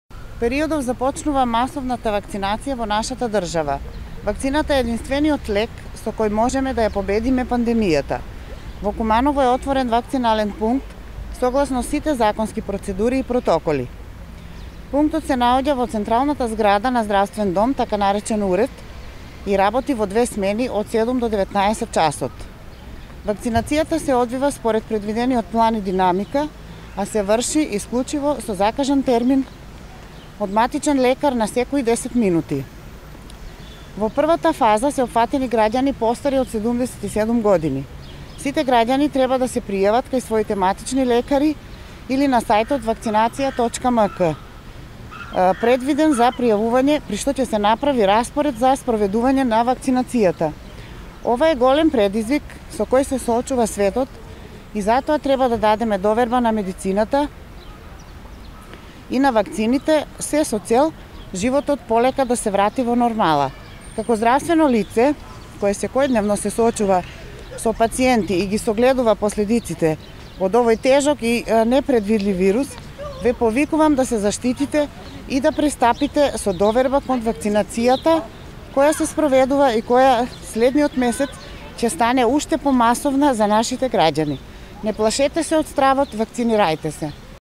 Прес оддржа СДСМ
Од партијата СДСМ, денеска во Куманово се огласија со соопштение во кое се повикуваат граѓаните на Куманово да пристапат со доверба кон процесот на вакцинацијата како би можеле сите да се вратиме што поскоро во нормала. Останатите детали за тоа каде и со која динамика се врши имунолошкиот процес во Куманово.